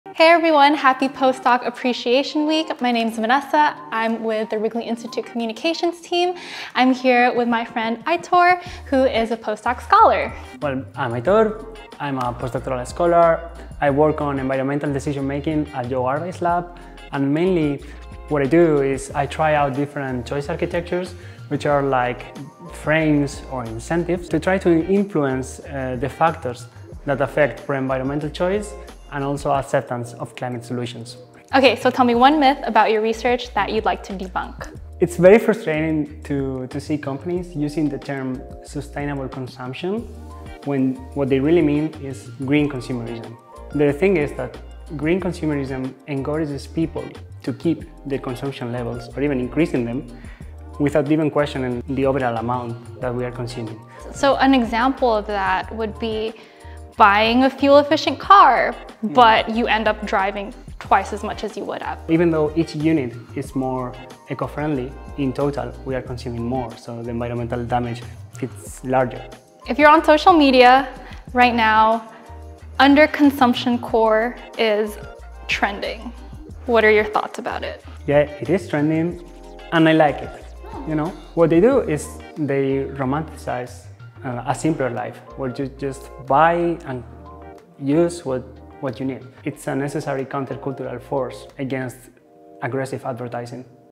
National Postdoc Appreciation Week with USCWrigley's Aitor Marcos Diaz
We're celebrating National Postdoc Appreciation Week with USCWrigley postdoc Aitor Marcos Diaz, who studies consumer behavior and ecological economics.

He’s exploring big questions around green consumerism (choosing eco-friendly products) vs sustainable consumption (reducing overall resource use). Plus, hear his thoughts on the underconsumption trend taking over social media!